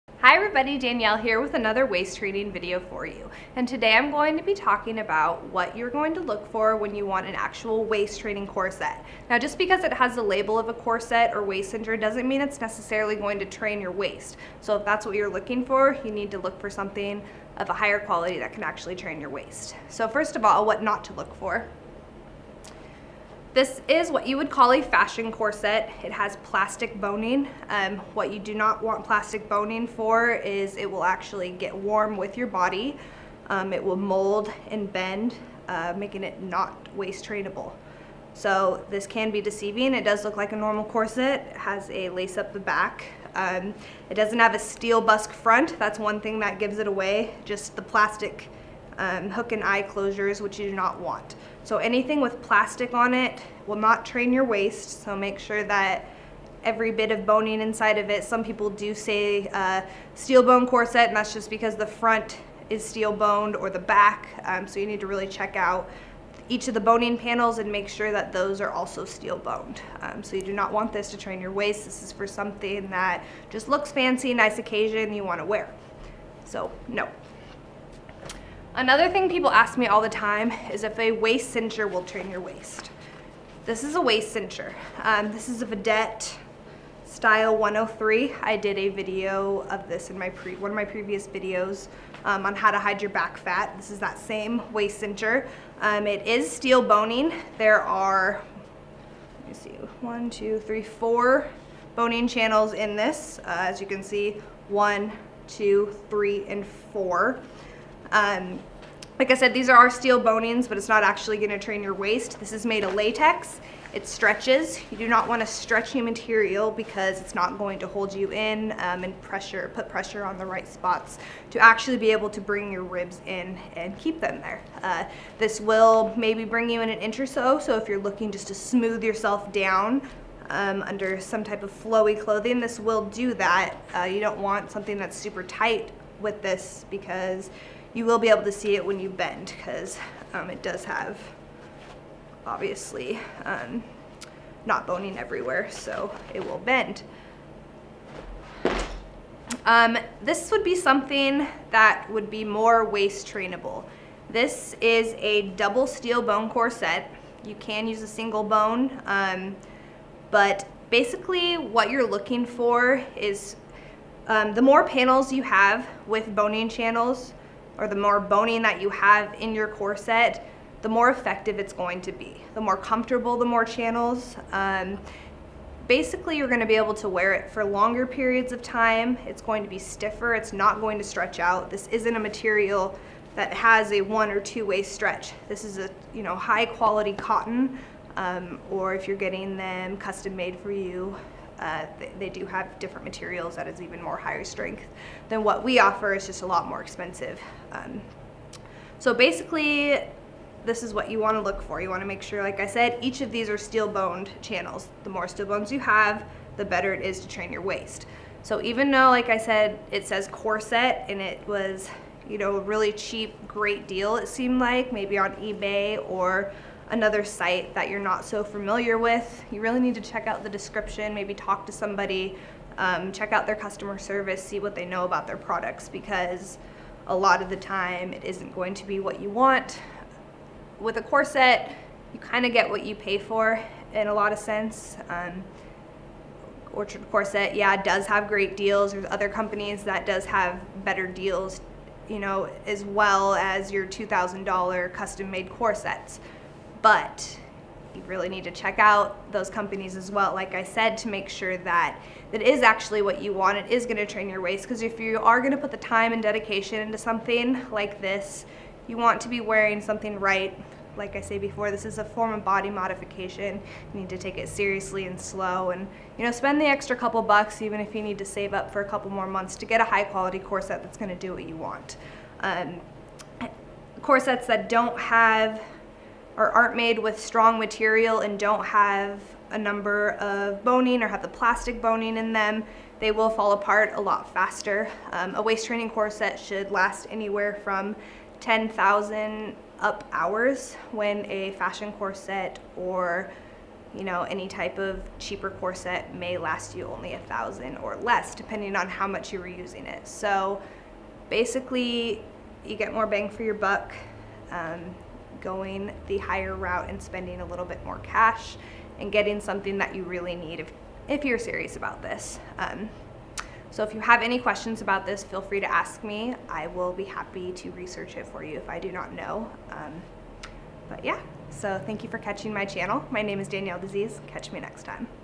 How to Waist Train: Episode 8: Can I Waist Train in This?
This video I go into detail about what you can and can not waist train in, and also what you will want to look for when searching for a proper "training" corset.

♡ Social Media ♡

♡ Some Favorites! ♡